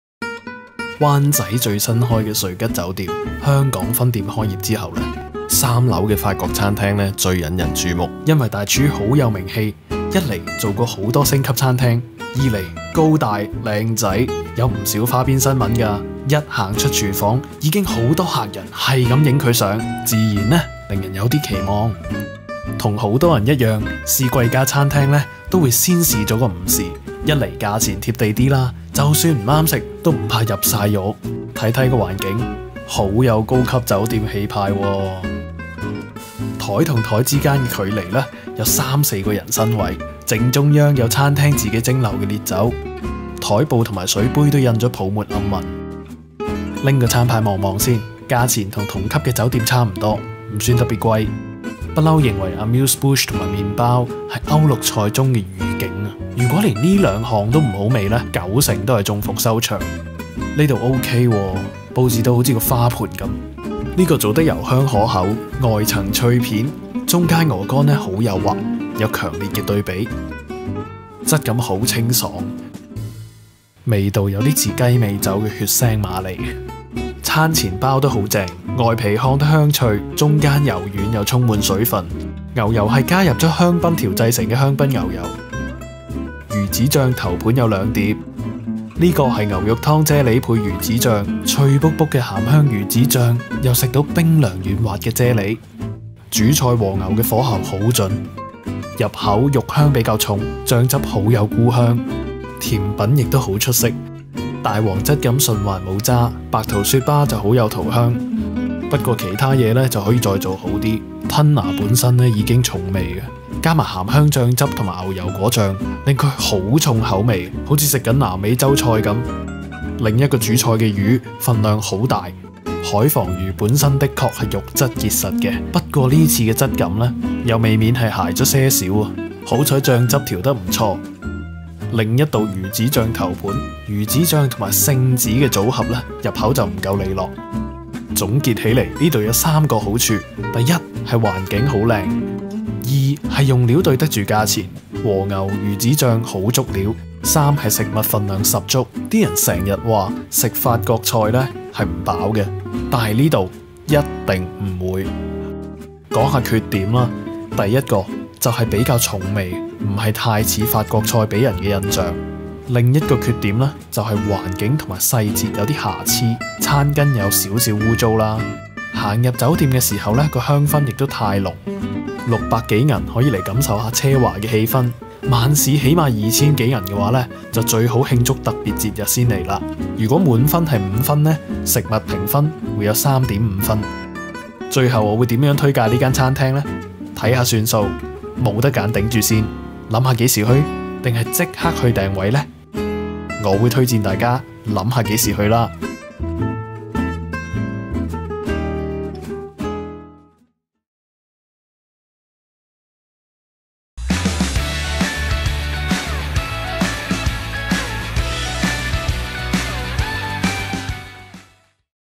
【灣仔法國菜】$588 5道菜午餐 起食18粒星名廚手藝 A3和牛肉香味重 環境有氣派
灣仔瑞吉酒店一向標榜貴族式服務，香港分店開業後，已吸引不少闊太光顧。4間餐廳酒吧中，二樓的法國餐廳L'Envol最引人注目，因為L'Envol請來曾主理多間星級法國餐廳如Pierre Gagnaire、L’Atelier de Joël Robuchon Hong Kong、Seasons by Olivier E的大廚Olivier Elzer當餐廳大廚，在維基百科被頌揚為擁有18顆星的名廚，自然令人有高期望。

不少人應該都和我一樣，試貴價餐廳時，先試一次午市，一來價錢較親民，即使水準差，也不會太傷錢包，所以這次就試了5道菜午餐，再單點了一個魚子醬頭盤，看看水準如何。

未進餐廳，先感受酒店的一對一服務。酒店大門有專人開門，電梯大堂亦有專人問顧客到口中哪層樓，然後為顧客按動電梯，到了餐廳，亦立刻有接待員現身，顧客一坐下，亦立刻有職員上前問好，以香港情況而言，員工和顧客比例的確是難得的奢侈。不過顧客入酒店時，所聞到的香氣實在太濃重，令人覺得有點窘息。

環境相當令人滿意，空間感足夠，樓底高，枱和枱之間的距離有三四個人身位，地毯厚綿無聲，餐枱和餐椅間不會過窄過寬，佈置簡約而不簡陋。餐廳當眼處還擺放了自家蒸餾的天藍色烈酒。所用餐具如餐碟、餐刀都是法國出品，枱布及水杯上，都印有泡沫暗紋，暗示大廚熱愛的香檳氣泡。整體佈置能令人留下深刻印象，感覺摩登。

菜式水準如何呢﹖餐廳提供廚師套餐及十多款單點菜式，午餐提供三款套餐，收費$588/3道菜，另外有$698/4道菜，以及$798/5道菜，晚餐就是7道菜晚餐為$1,788，另有十多款單點菜式，$528的法式海龍蝦至$1,788的法式原隻燒雞。

乍看價錢並不便宜，但細心一想，區內鄰近酒店午餐，其實絕大部分都收這個價錢，環境或菜式數目甚至比不上L'Envol。用便宜來形容L'Envol就未免離地，但比同級餐廳，L'Envol就頗為相宜。

個人一向認為Amuse Bouche及麵包是歐陸菜中的預警，假如這兩項失色，基本上餐廳已是大勢已去。L'Envol的Amuse Bouche搶眼，佈置如花盤，可食用沙律菜、小蘿蔔放滿石盤中，另外有一道番茄啫喱，晶瑩剔透，整體賣相出色。薑糖鵝肝油香可口，外層脆片和中心幼滑鵝肝有強烈對比；另一個是雅枝竹配炸紅葱，質感很清爽；番茄啫喱入口冰涼，味道有點像雞尾酒中的血腥瑪莉，不過就沒有了辣味。麵包極出色，外皮烘得香脆，中心柔軟又充滿水分，牛油是加入香檳調製的香檳牛油，入口油香軟滑，味道帶點果酸味，和麵包配搭起來，很美味，令人對接下來的菜式很有信心。

先說魚子醬頭盤，一共分成兩項，其中一款是個人心水，大廚選用法國魚子醬Shadi，下層以牛肉湯再凍成啫喱，入口先嘗到脆卜卜的鹹香魚子醬，再嘗到冰涼軟滑的啫喱，味道質感都形成對比，令人留下深刻印象。

主菜及甜品令人滿意，主菜和牛就合乎標準，選用A3和牛，入口有質感，肉香較重，配搭煮得入味的蘑菇，味道配合度相當令人滿意。甜品用大黃同馬鞭草等煮得入味，鮮酸開胃，大黃質感順滑無渣，是少有的出色，配搭白桃雪葩，一甜一酸，平衡度剛好。

其他菜式就有爭議。如第二頭盤的五香吞拿魚配牛油果醬及脆紅葱，煎得外層微熟的日本吞拿魚，下層是牛油果醬，吞拿魚魚味重，加上鹹香醬汁及牛油果醬，令這道菜味道相當重口味。個人認為其實味道不錯，非常惹味，但不是法國菜應有的優雅風味，反而很有墨西哥或南美的熱情，同枱就有人覺得過鹹，非常視乎個人接受程度。

另一主菜的烤海魴魚，海魴魚本身是肉質結實的白身魚，的確不該要求滑溜，但魚肉質感又未免太嚡了，魚味亦偏淡，幸好配搭的紅酒汁調得相當好，不會單調。另一道魚子醬頭盤，以魚子醬配搭蟶子，個人就認為調味有點失準，魚子醬和蟶子的組合亦有點糊口。個人認為另一道魚子醬頭盤出色多了。

總括來說，L'Envol環境出色。食物風格符合近年的摩登法國菜風格，用日本食材如吞拿魚、和牛來組合菜式，用料好，菜式分量大，算是很抵食。缺點是調味偏重，鹹、辣等口味都濃重，如果和同級又同樣是新開業的法國餐廳比較，就欠了點細膩。

服務方面，侍應效率高，很注意顧客動向，笑容令人滿意，就是細節位仍有進步空間。如顧客所用的餐巾有少許污迹，在這種級數的餐廳實在不該出現。還有，職員問顧客是否需要放置背包於儲物室內，禮貌周到，但顧客取回背包時，發現職員把同枱友人的一部手提電腦放了進背包，而沒有事先得到允許，這點很容易引起爭拗及顧客不滿，需要多加注意。

以入門價$588的三道菜午餐，可以嘗到這間人氣法國菜名店，以近年消費而言，算是相當抵食，晚市平均消費達$2,000，以環境、菜式用料及分量而言，其實也可以接受。如果周年紀念活動想試新餐，或覺得中環、金鐘等同類酒店法國菜實在太貴（晚市起碼超過$3,000），來L'Envol並不會後悔。

編輯、攝影：嘴尖尖

L'Envol
地址：灣仔港灣道1號香港瑞吉酒店2樓